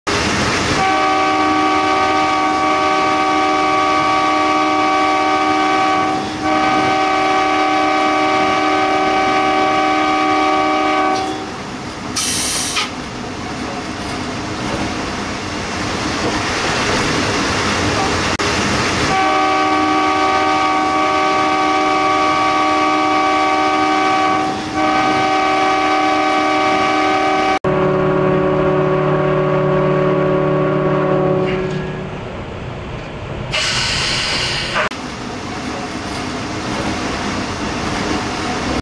台鐵 枋寮路段 兩隻小狗誤闖軌道驚魂
2018.2 台鐵 枋寮路段 兩隻小狗誤闖軌道驚魂